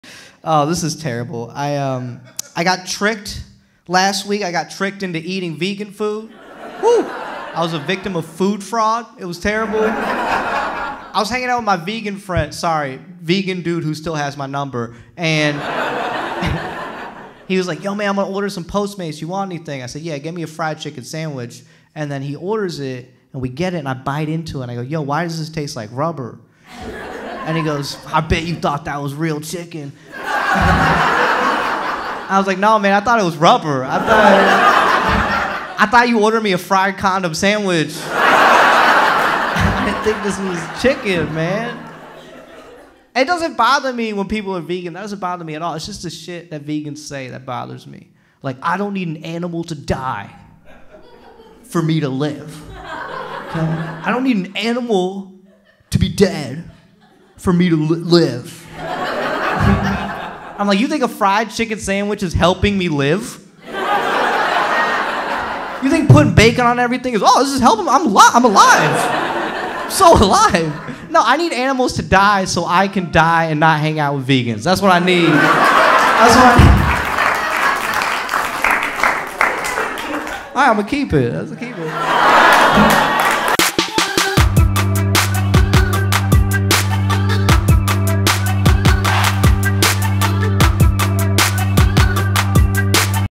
Vegans are DIRTY TRICKSTERS | Neel Nanda | Stand-up Comedy
Neel Nanda performs stand-up comedy about vegans. This was performed at the Broadwater Theater in Los Angeles, CA. This was the first time I had headlined a show in LA. This is/was a new joke which is why I say "keeping it" at the end. Can't believe a joke about hating vegans got an applause break in Los Angeles, that's why I had to upload this clip.